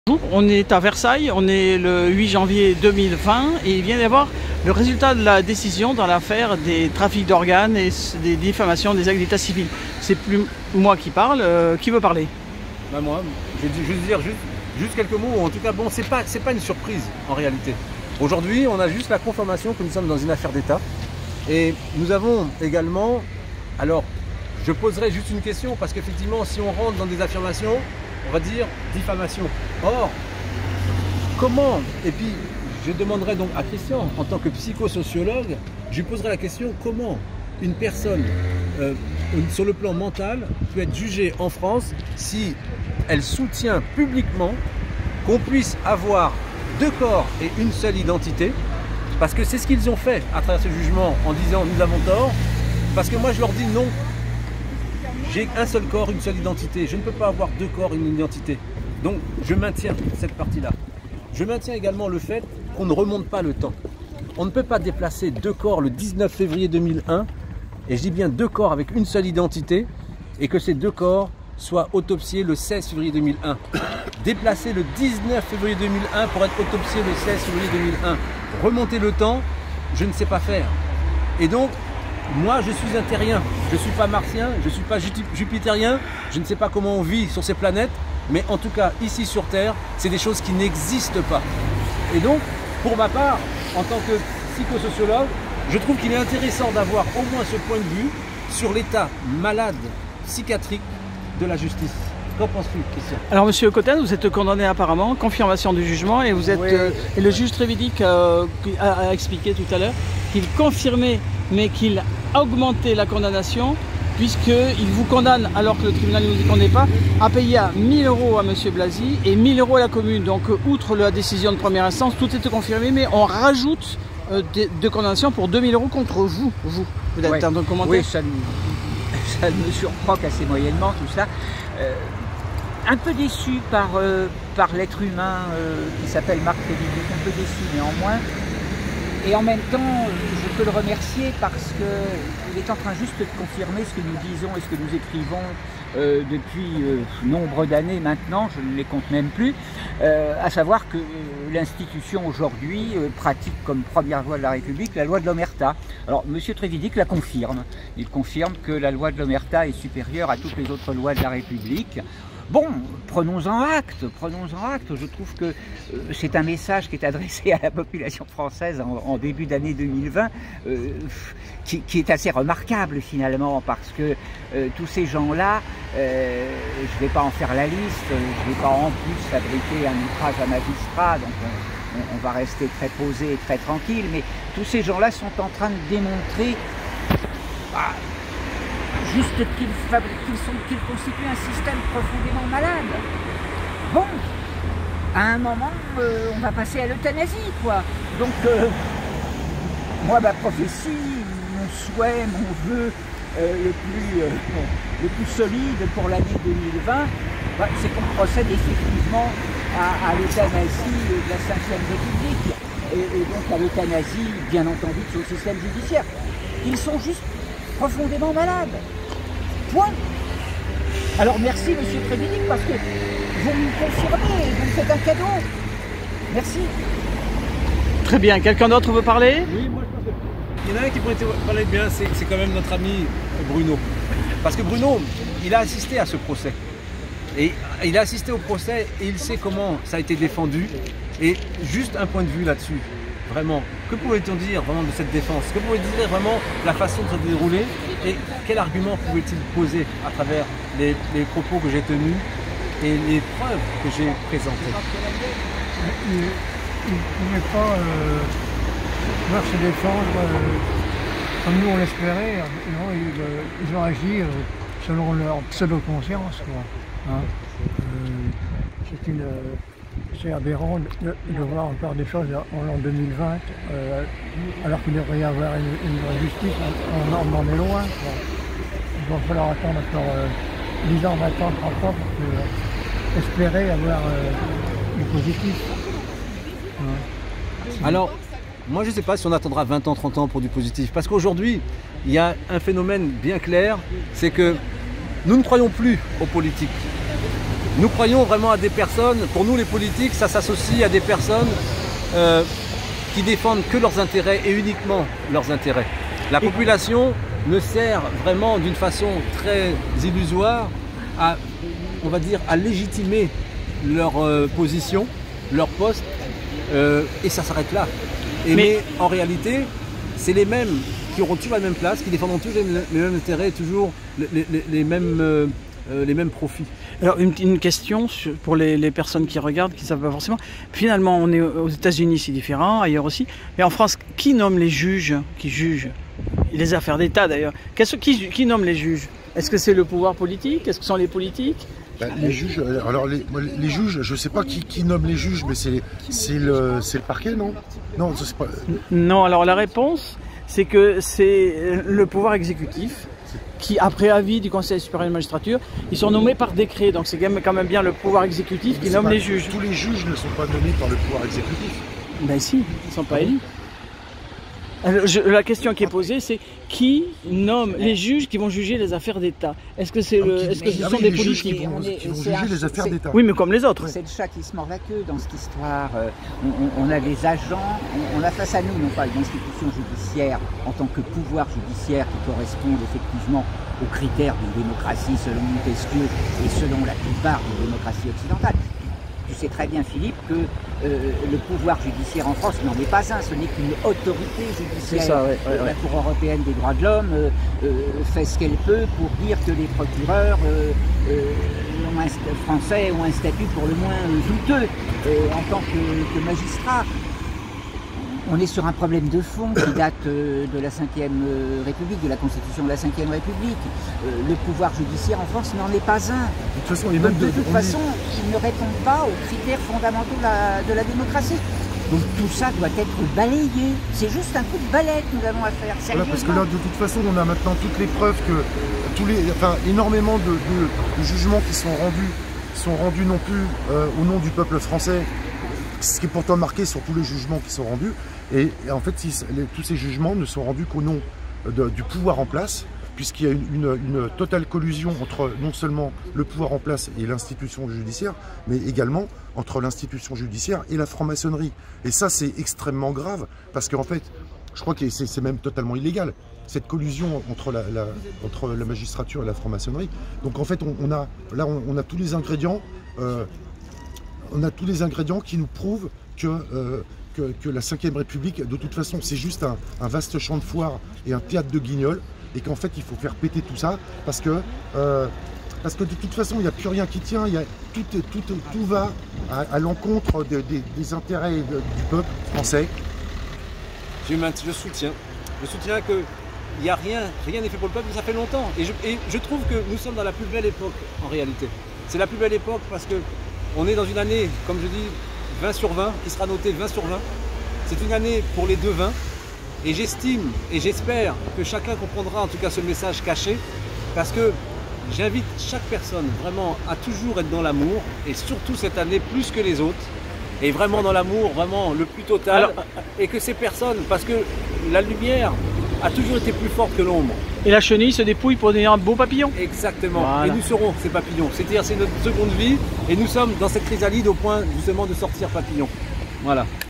La Cour d'Appel de Versailles condamne Thierry Kabile et Christian Cotten. 8 1 20.
Maître Philippe FORTABAT-LABATUT, Thierry KABILE, Christian COTTEN et nos deux amis Yves CHOUARD et Bruno commentent la décision rendue ce jour 8 janvier 2020 par la Cour d'Appel de Versailles. La justice continue à se décrédibiliser en continuant plus que jamais à appliquer la loi de l'OMERTA pour protéger des acteurs publics auteurs de crimes en bande organisée.